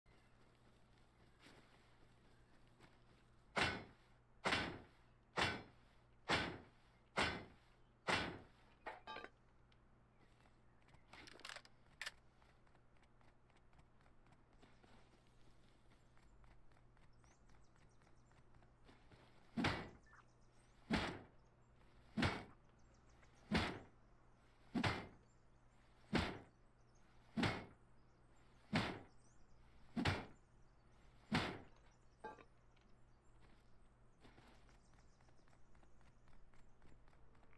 dont forget about these
i hardly ever see anybody farming these and there are so many of them along the road sides and at outpost